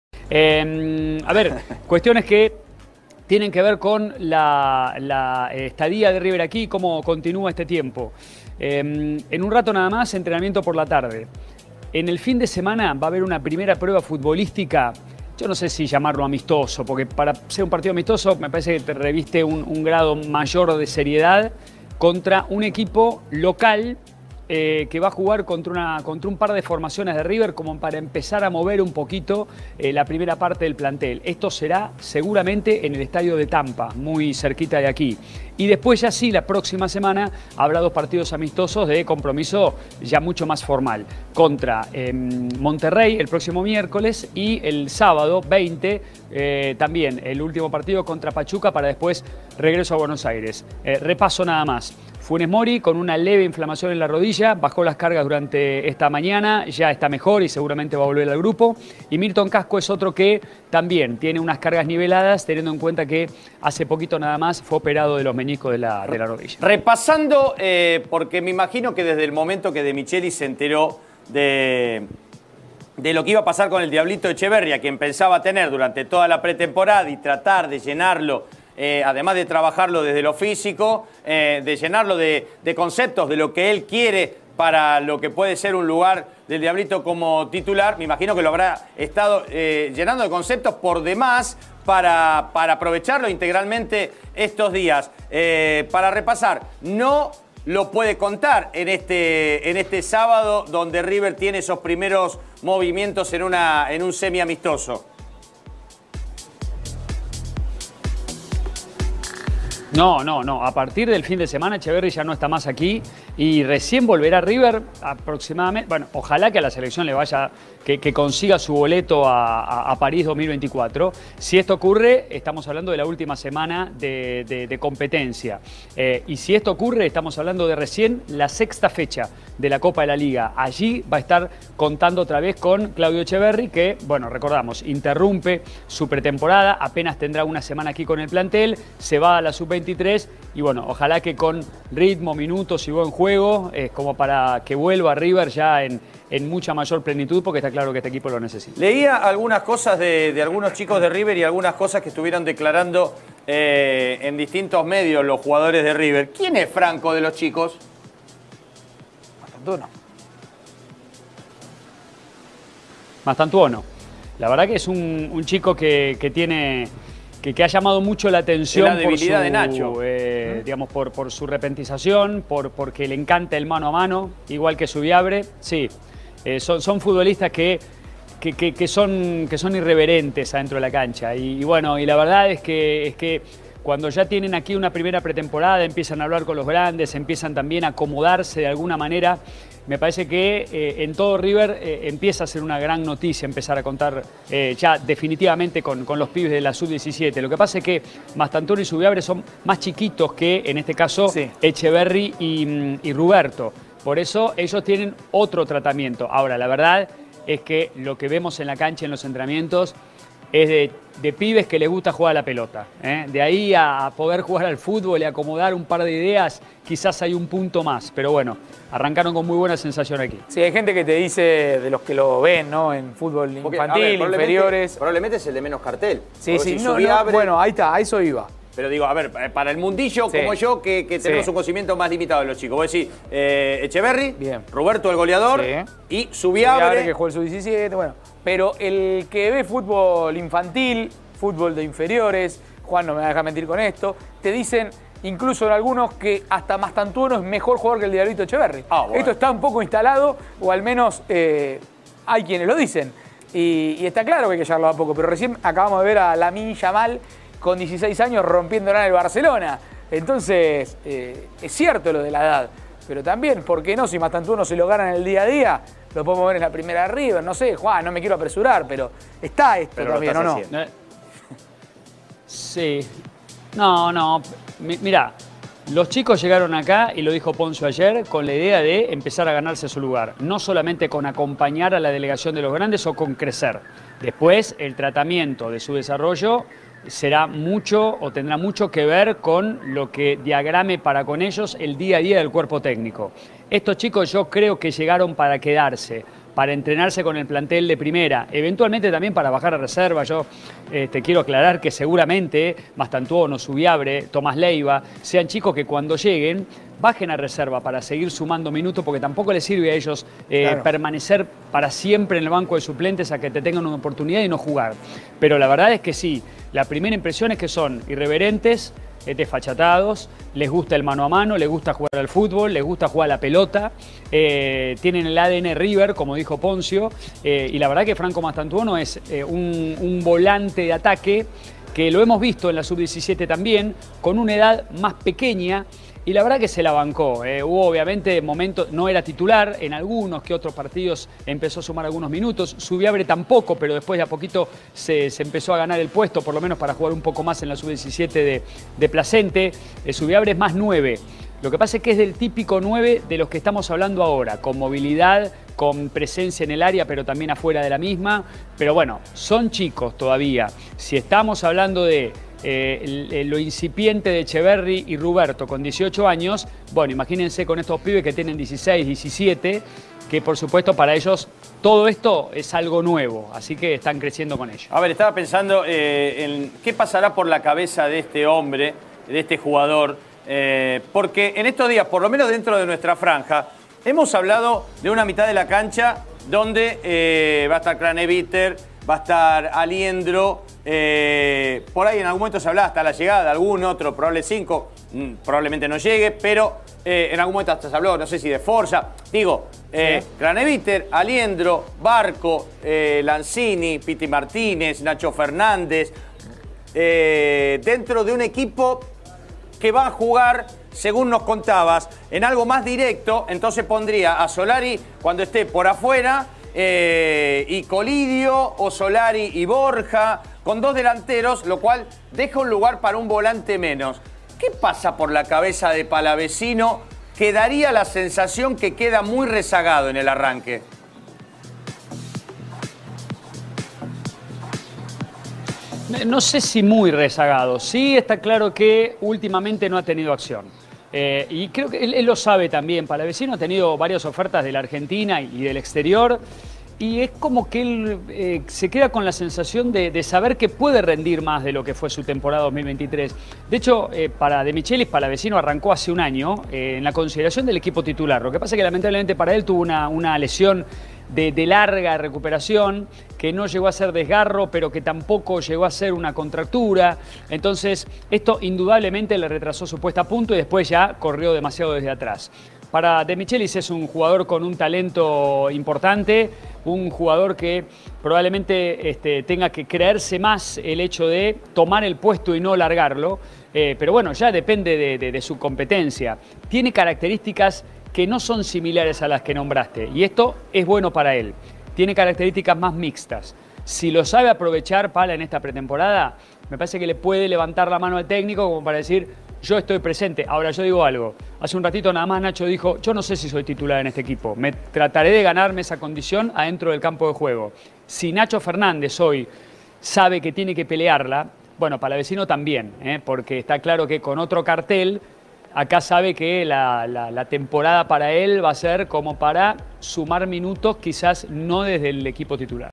PERIODISTA ARGENTINO AFIRMA que MASTANTUONO es MEJOR que ECHEVERRI
Cortese afirma que River no tiene de que preocuparse, las promesas de River están con la flechita para arriba y asegura que Mastantuono es mejor que Echeverri.